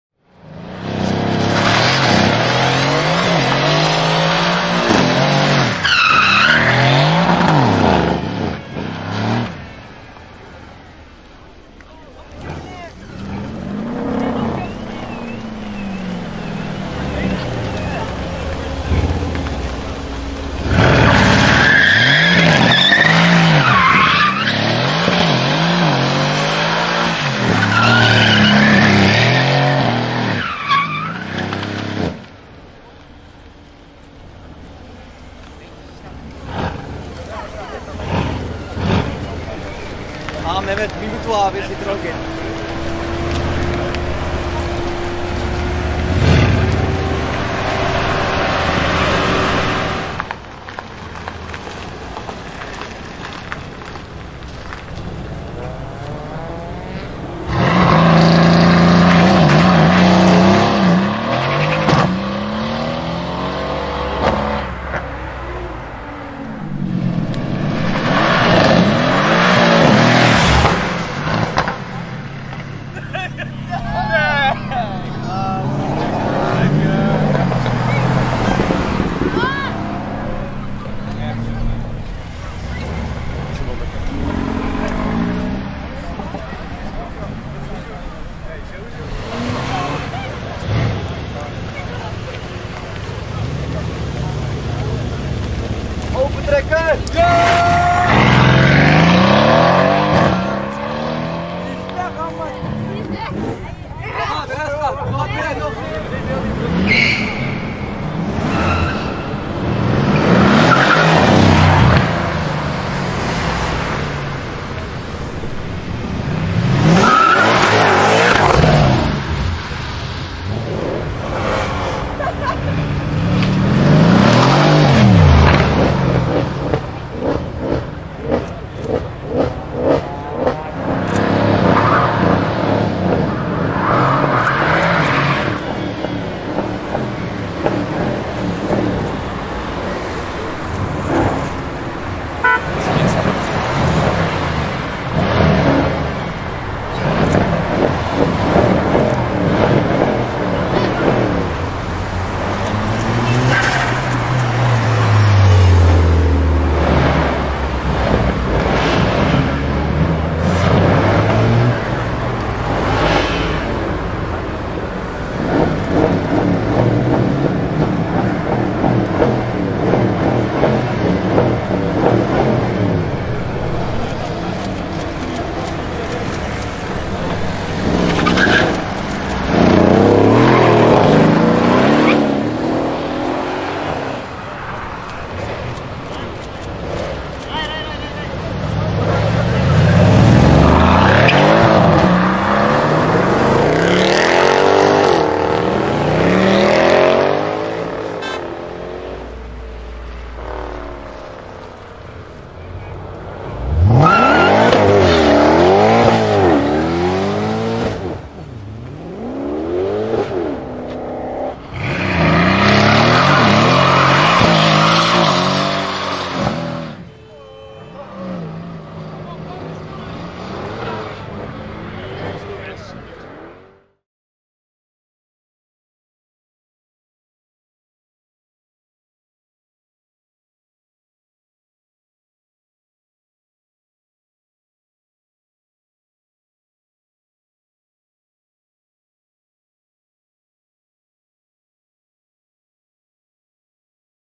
Mercedes C63 S AMG - Donuts, Burnouts, Launch Control & Stopped by Police !
Yesterday we visited a supercar event called Dutchcartour2017. At this event we saw this Mercedes C63s AMG making some donuts, a launch control and a burnout. After doing that burnout he got stopped by the police, but luckily he didn't got fined. We also saw this C63s during Cars and Coffee Dordrecht where he also made some burnouts.